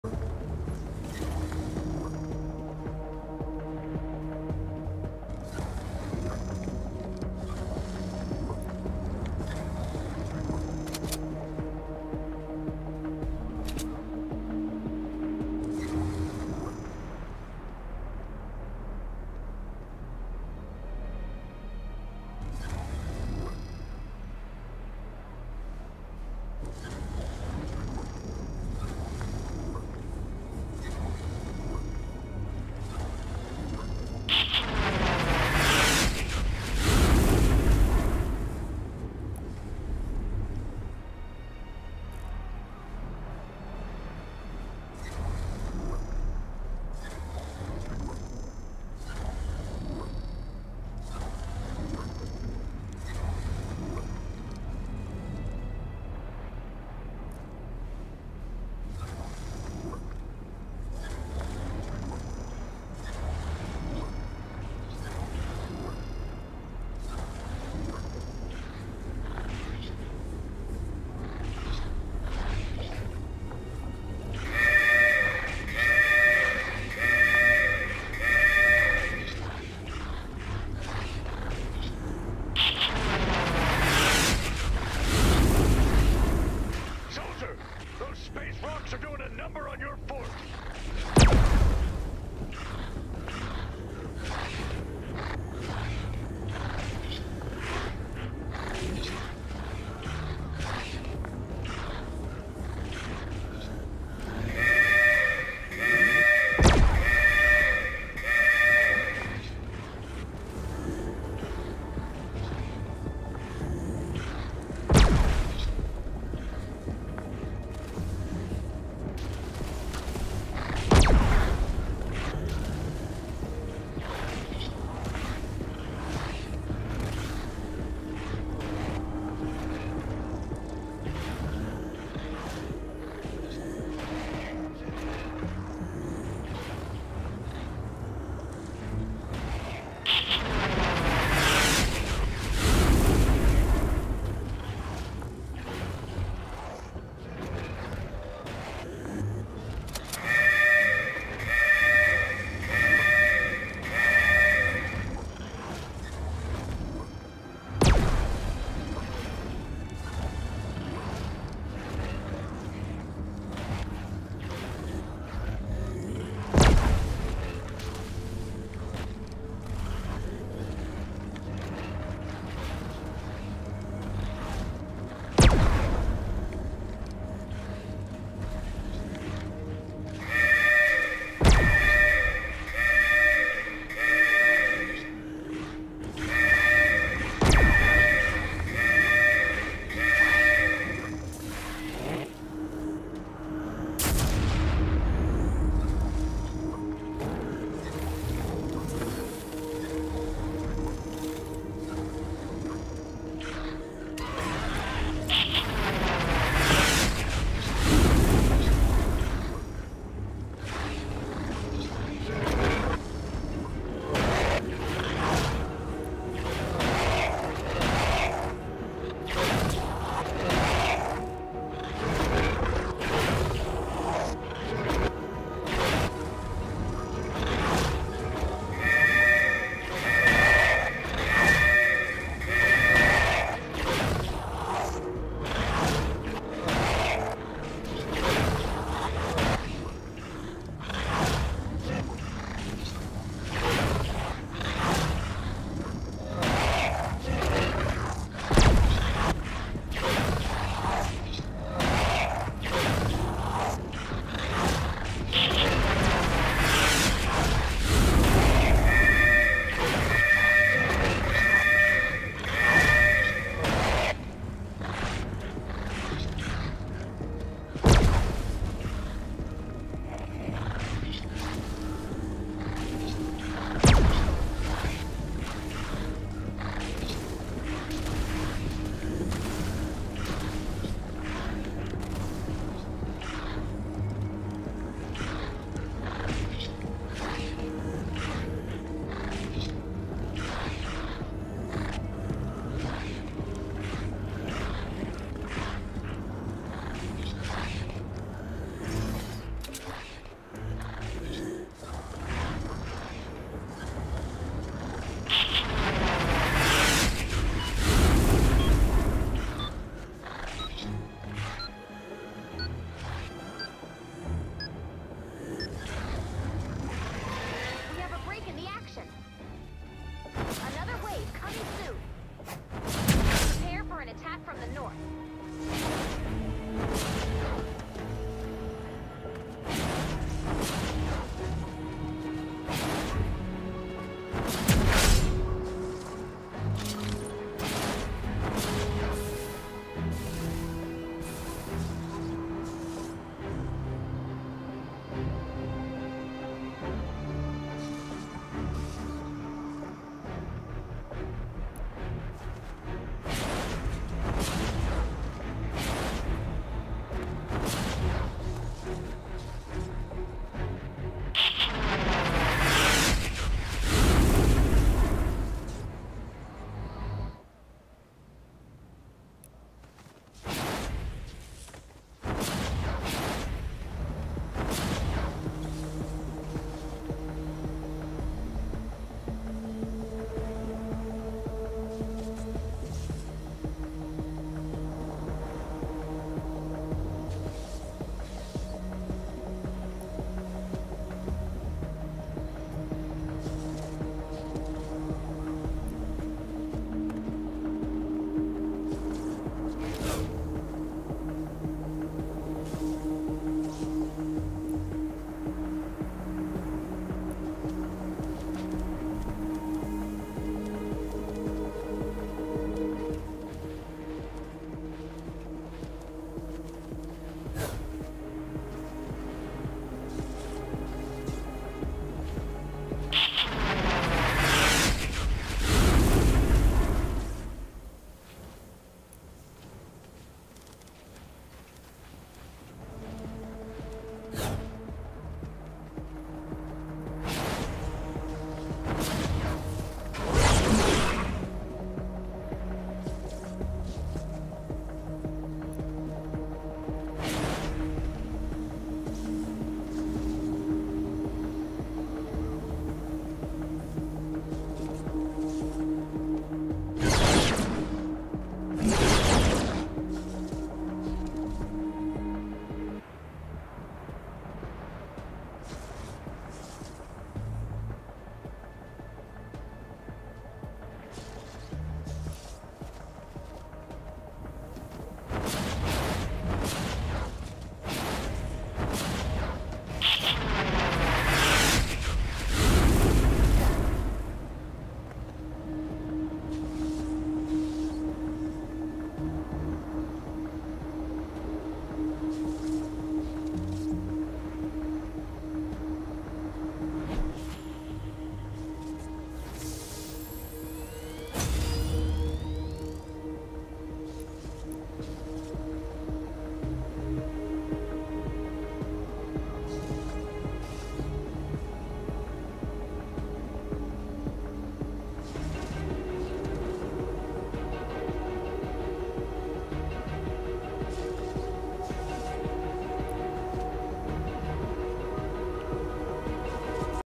Homeless Challenge (no walls/no floors) Bridge w20 And showcase after wave ends
Whitewalker community challenge "Homeless"  no floors, no walls!